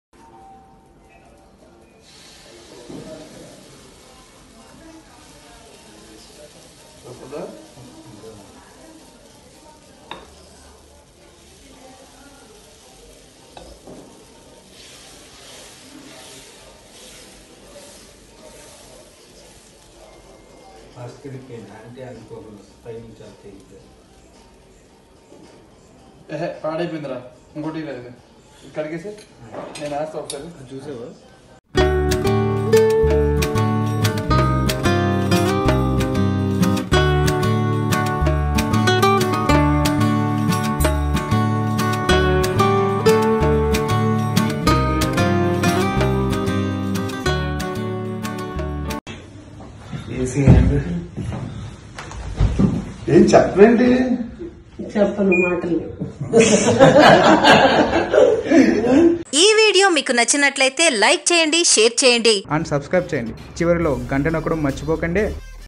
Actor Naga Shourya Making dhosa For His Mother | Naga Shourya Cooking | Life Andhra Tv
Watch here Actor Naga Shourya Making dhosa For His Mother